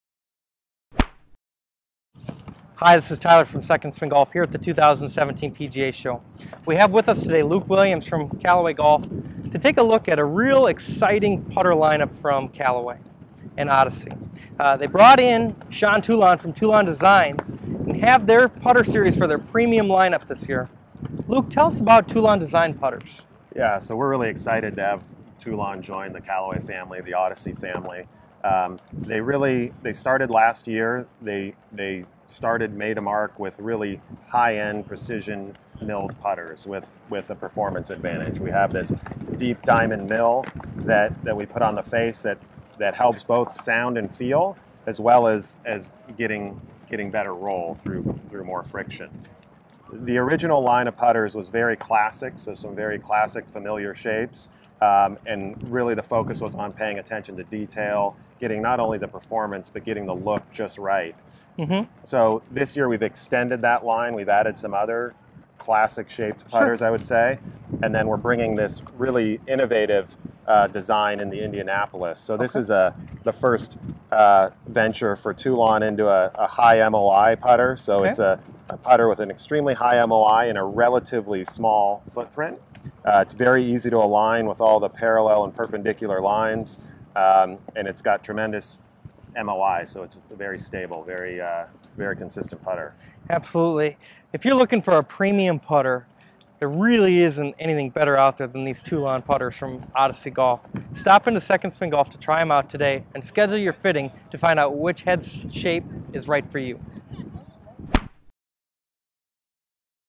Toulon Putter Line Review
Callaway and Odyssey Golf is pleased to announce a new partnership with Sean Toulon and Introducing a new line of Toulon Putters. Luke Williams the Senior Director of Global Product Strategy at Callaway Golf gives 2nd Swing a sneak peak at the new putters.